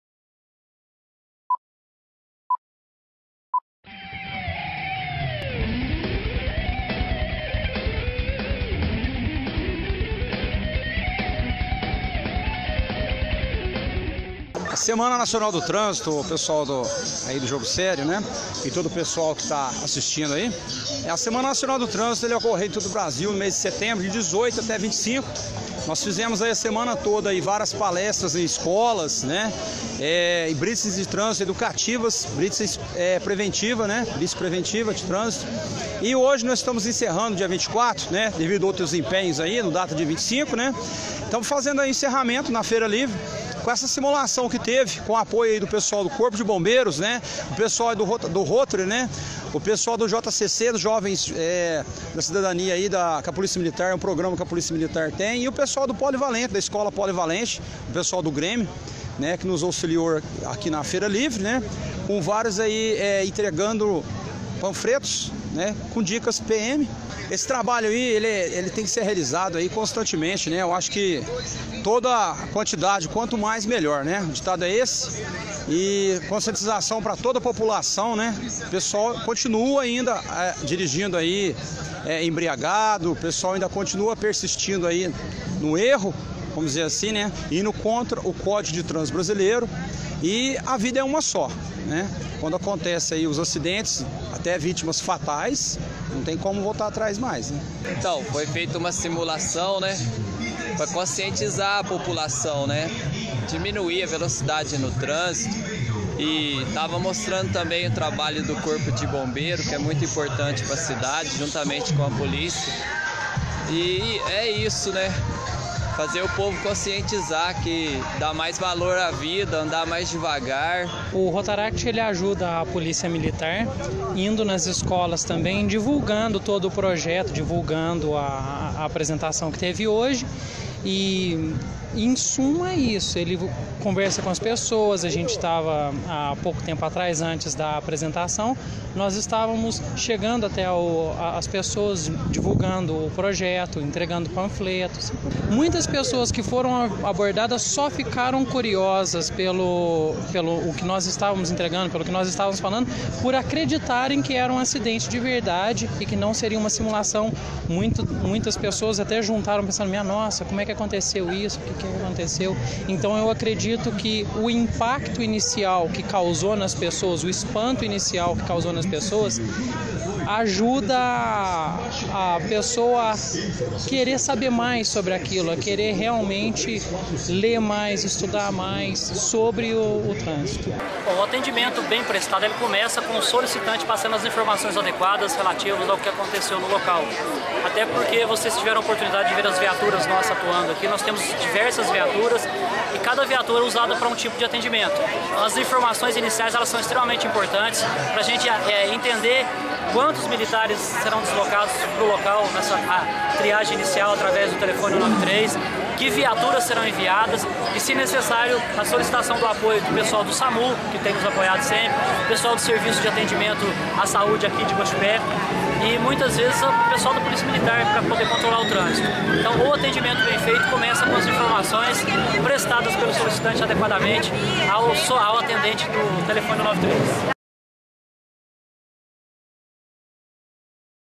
Corpo de Bombeiros e Polícia Militar realizam simulação de acidente na feira de Guaxupé
Na manhã deste sábado, 24 de setembro, a Polícia Militar, juntamente com o Corpo de Bombeiros, o JCC, o Rotaract de Guaxupé e a Escola Estadual Doutor André Cortez Granero (Polivalente), realizou uma demonstração de atendimento à vítima de acidente de trânsito.

Foi demonstrado como é feito o socorro de uma pessoa, a qual teria sofrido um acidente automobilístico por estar transitando na contra mão de direção e ter se chocado com uma motocicleta. No caso, a "vítima" ficou presa entre as ferragens, o que ocasionou o empenho de uma guarnição do Corpo de Bombeiros Militares, que realizaram o atendimento prévio do paciente, imobilizando-o e o retirando do veículo em seguida.

A ação faz parte da Semana Nacional de Trânsito, que ocorre desde o dia 18 último, a qual foi realizada pela Polícia Militar de Guaxupé, durante a semana, com a realização de palestras educativas sobre os perigos e deveres do cidadão no trânsito.